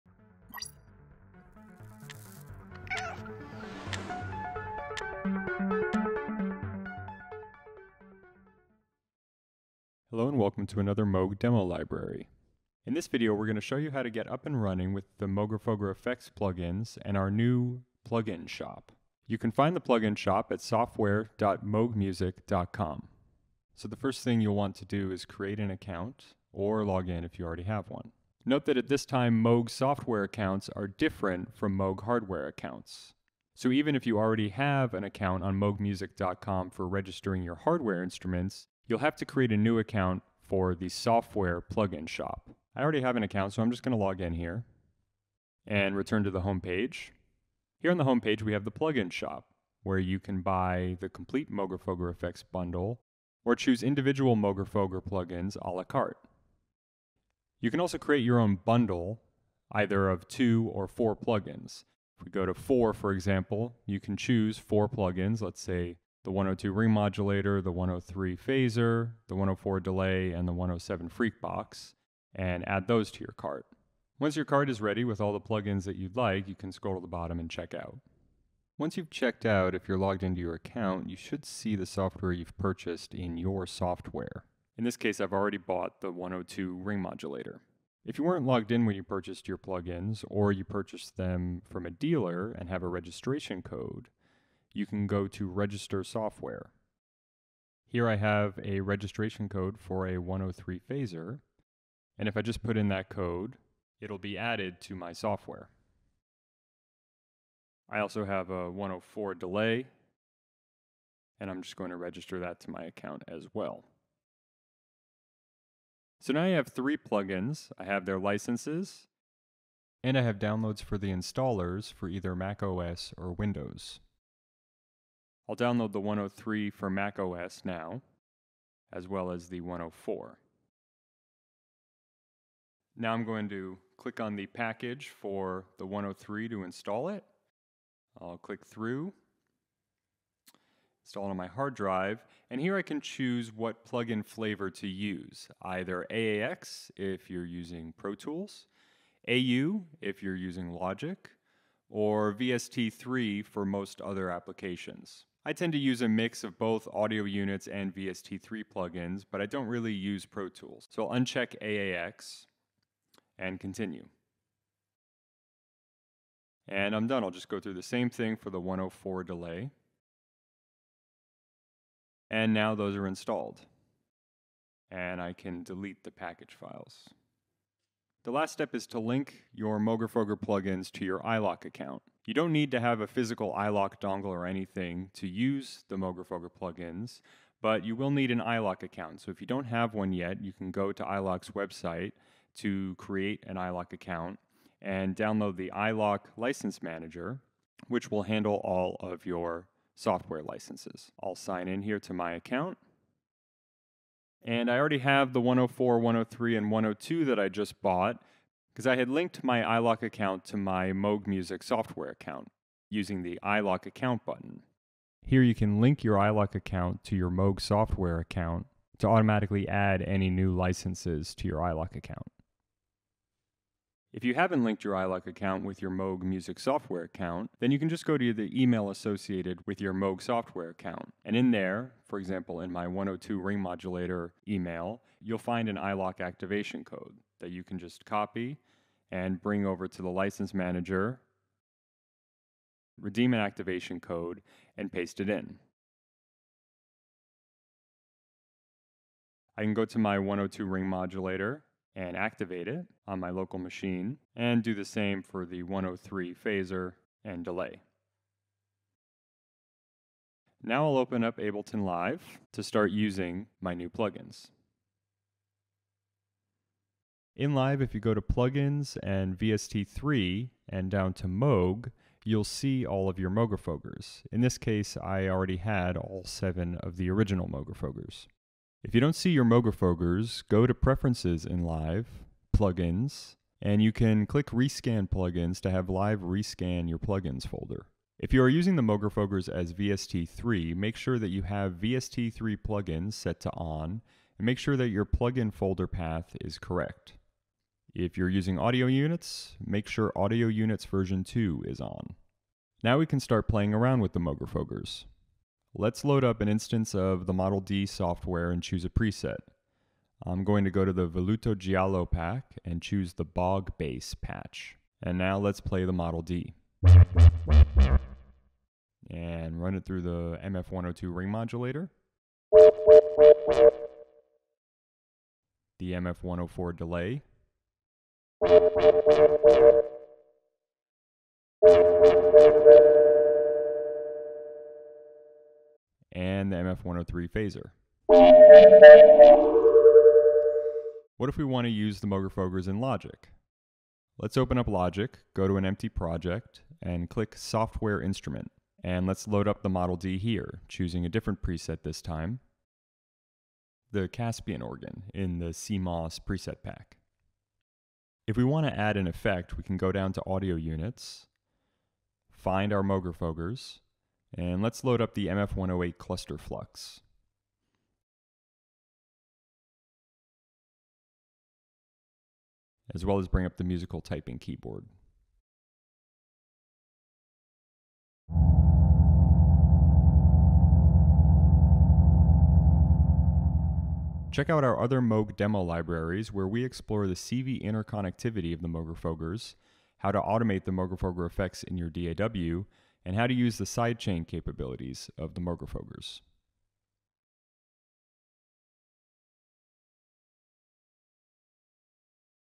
Moogerfooger Effects Plug-ins | How to Purchase, Register, and Install
In this Moog Demo Library video, we demonstrate how to purchase, register, and install your new Moogerfooger Effects Plug-ins. We then demonstrate how to load the plug-ins in both Ableton and Logic.